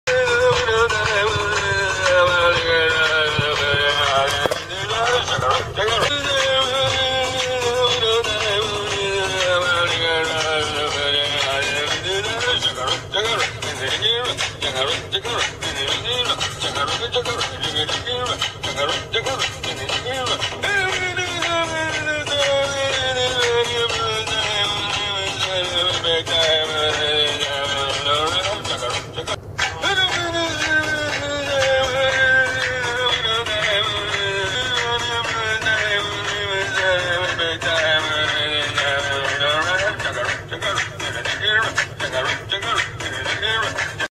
Batman on Drugs
I was just bored and fooling around. Doing a different twist on Batman on Drugs. Made good use of my face slimmer lips.